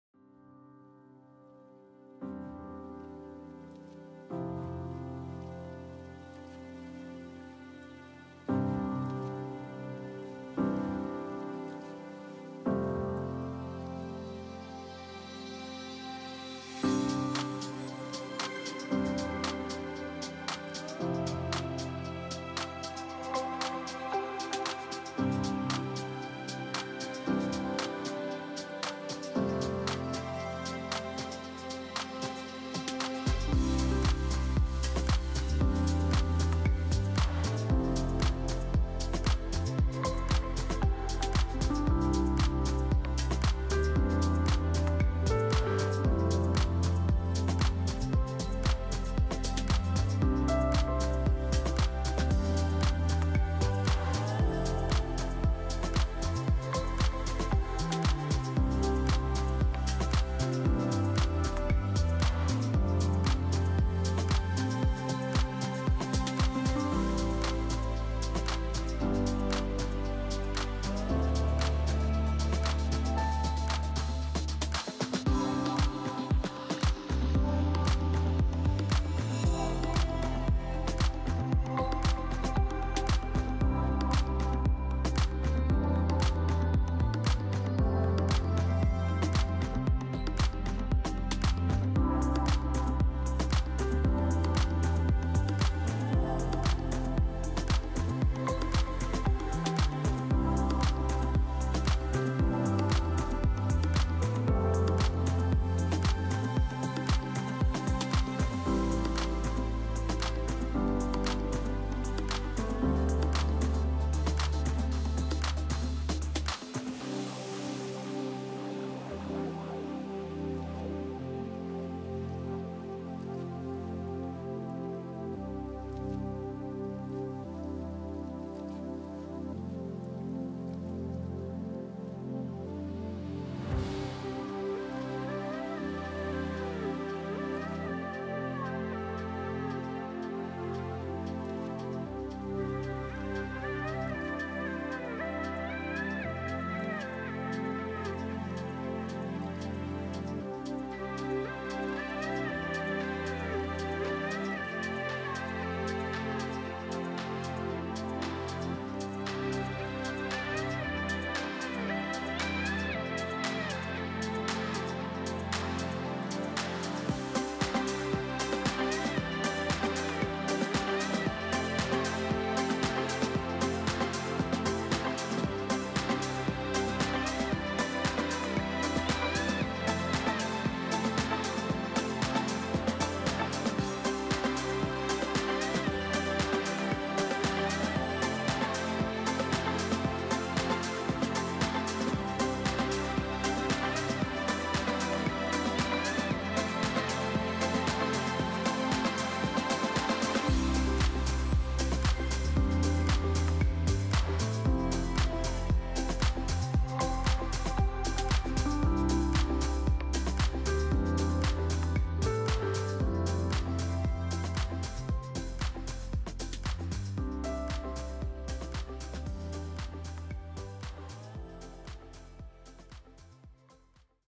Drone Tour of Eastside Construction
An aerial view of Anderson Farms, Sorrento Phase 2, Rancho Mirage Estates Phase 2, The Trails at Tortosa, and Tortosa South.